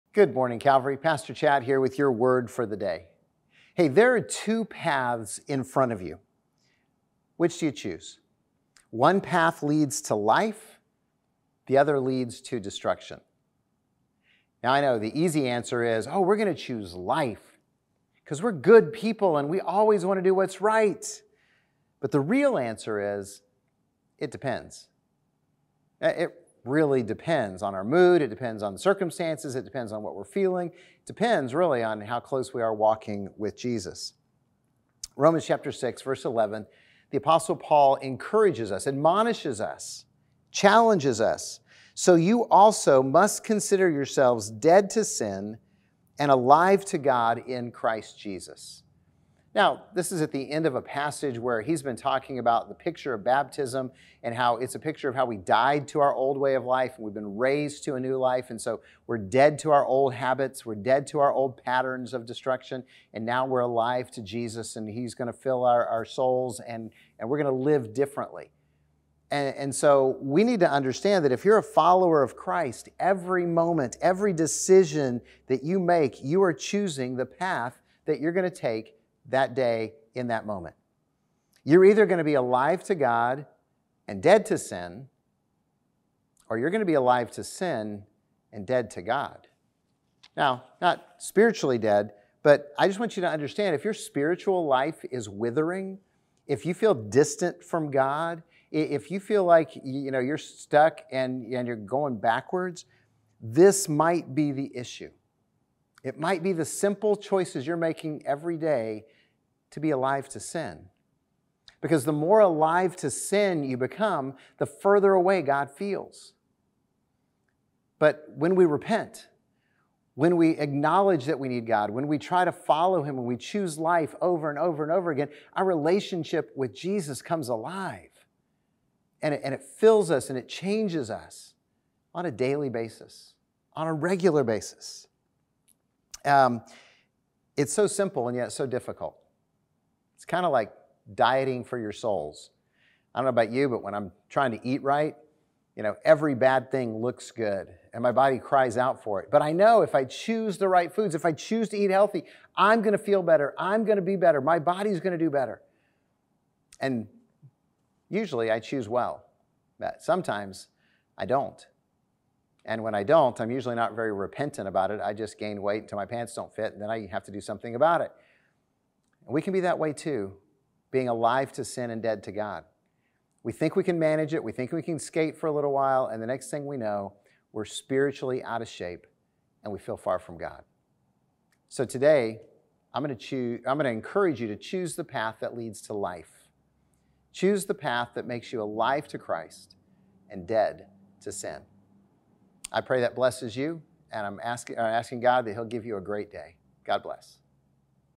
Your Word for the Day - Episode 706
Pastor Chad lays out the choice of paths available to us.